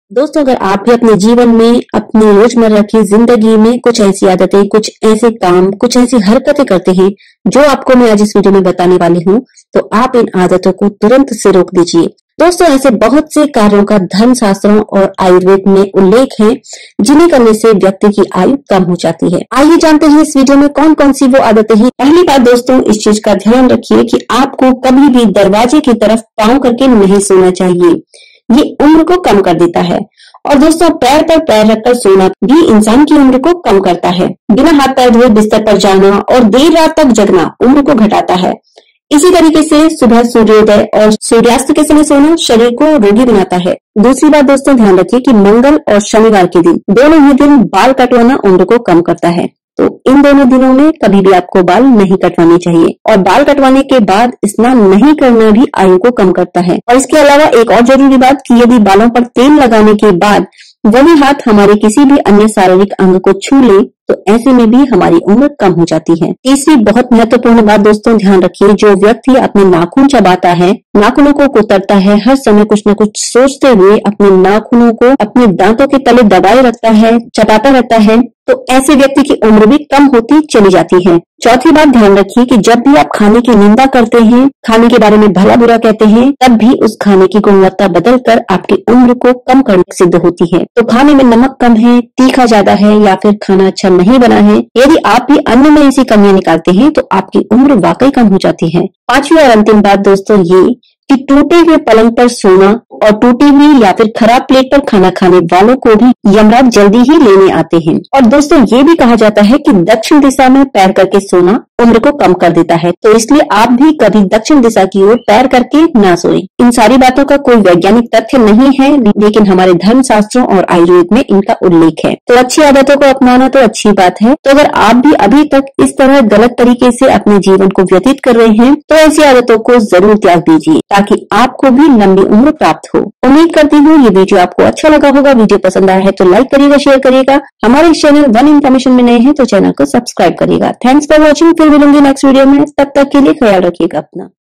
मंगलवार को बाल क्यों नहीं कटवाते हैं, दरवाजे की तरफ पैर करके सोने से क्या होता है || Do You KNOW
दरवाजे की तरफ पैर करके सोने से क्या होता है,मंगलवार को बाल क्यों नहीं कटवाते हैं,  Do You KNOW